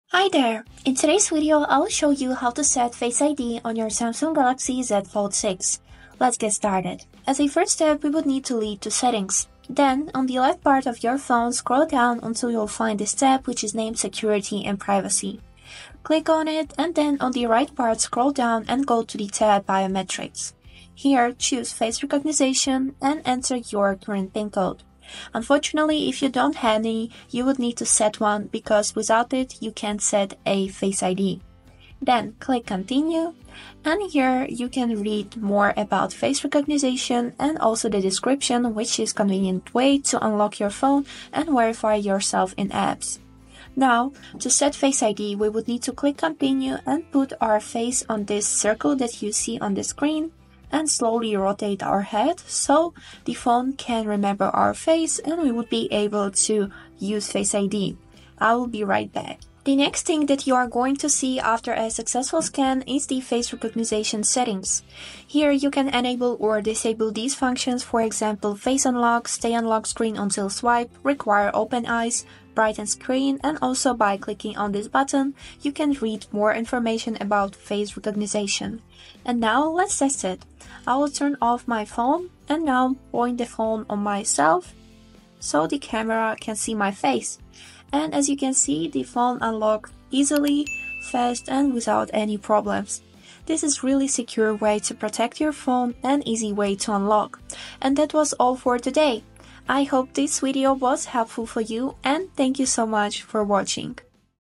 How to Setup Face ID Password on Samsung Galaxy Z Fold 6: Quick Guide
Want to secure your Samsung Galaxy Z Fold 6 with a Face ID password? This video will guide you through the easy steps to set up Face ID on your device, ensuring your data is safe and secure. Discover how to activate and customize Face ID settings to fit your needs, and make unlocking your phone faster and more convenient. Learn the best practices for optimizing Face ID functionality and protect your phone like a pro!